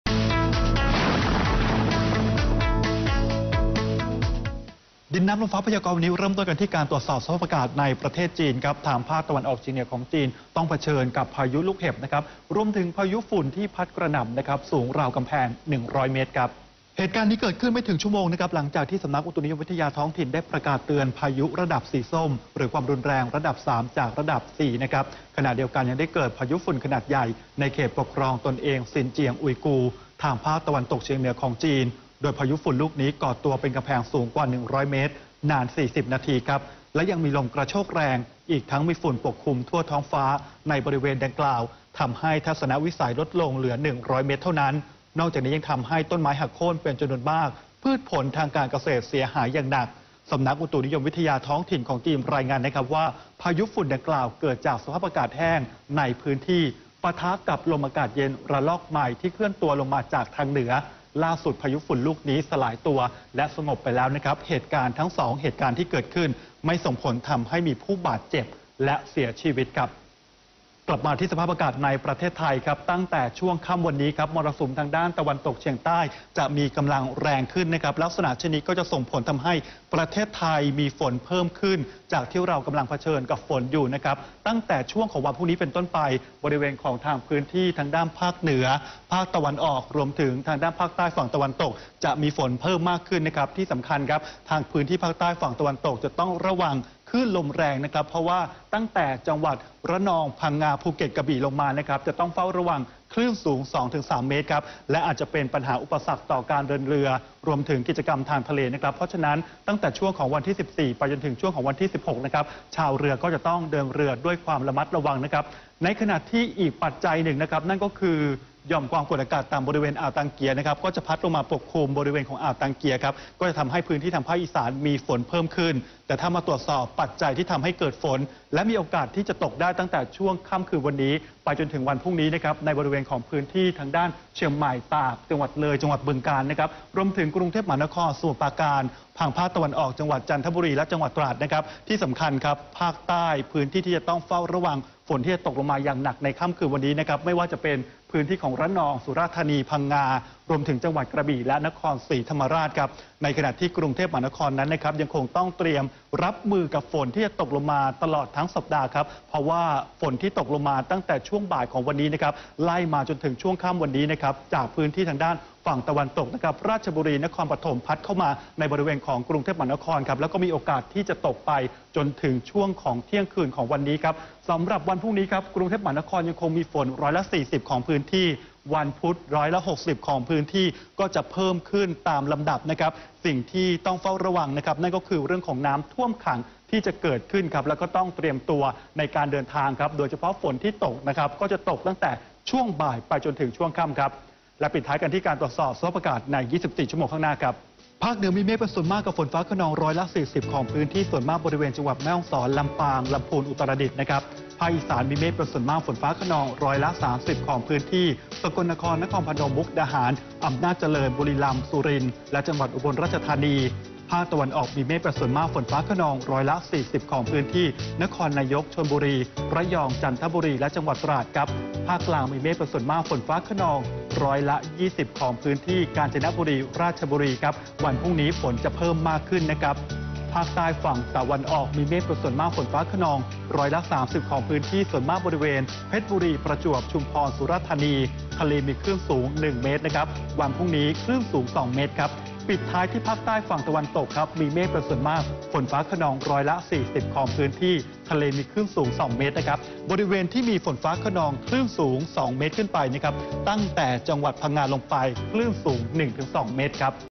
คนกรุงฯยังเผชิญฝนบ่าย-ค่ำ ตลอดสัปดาห์
ดินน้ำลมฟ้าพยากรณ์ : คนกรุงฯยังเผชิญฝนบ่าย-ค่ำ ตลอดสัปดาห์ "NOW26" 13-6-59